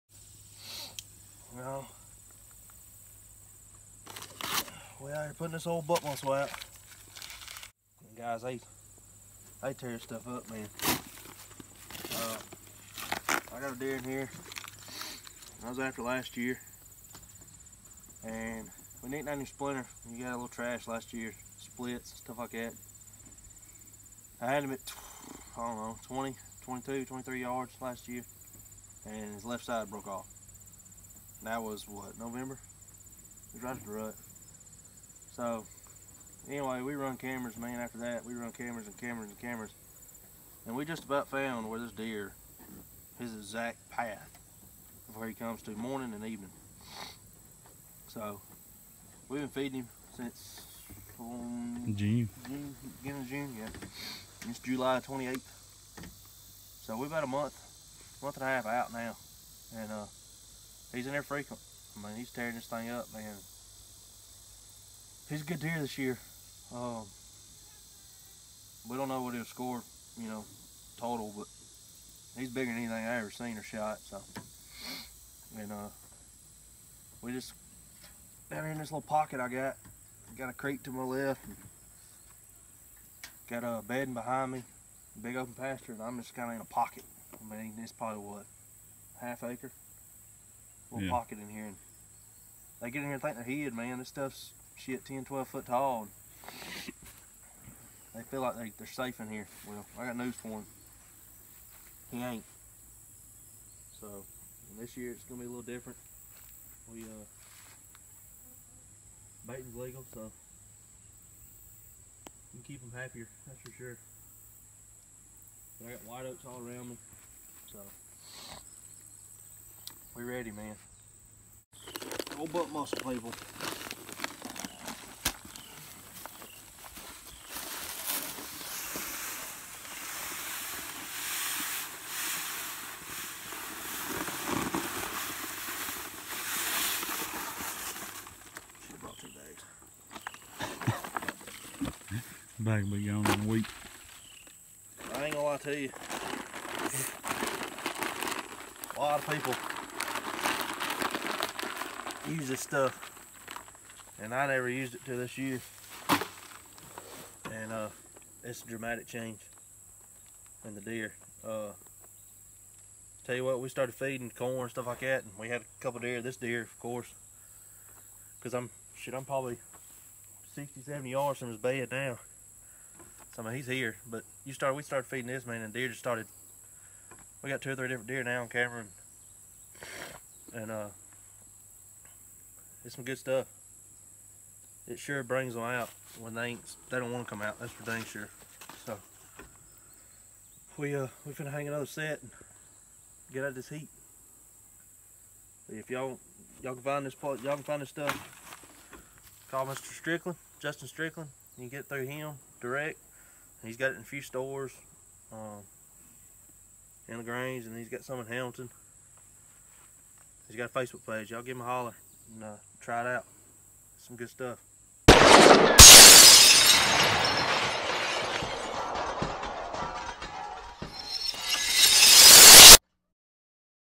The Hunt for Splinter
It's getting close to time for Georgia's opening day of archery season. Phillip has a potential 150 inch buck that hoping to have figured out by then.